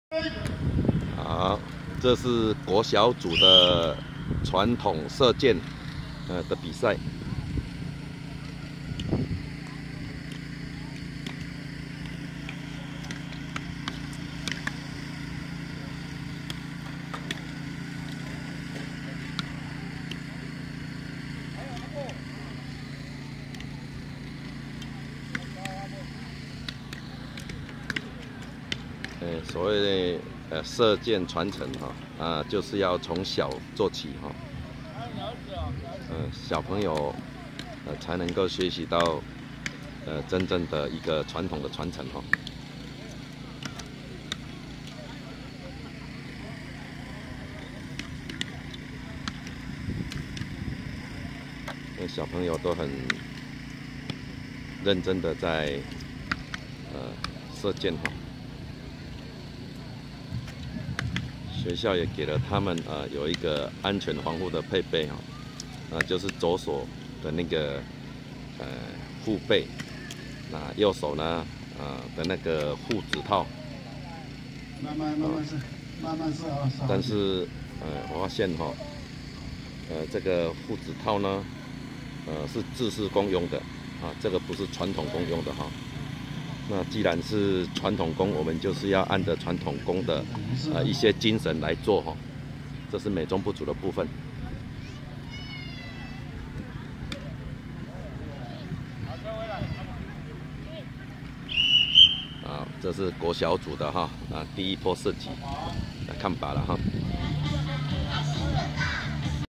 2015延平鄉第二屆鄉長盃全國布農族原住民傳統弓箭射箭賽-國小組
2015.7.5日(日)
台東縣延平鄉桃園村桃園國中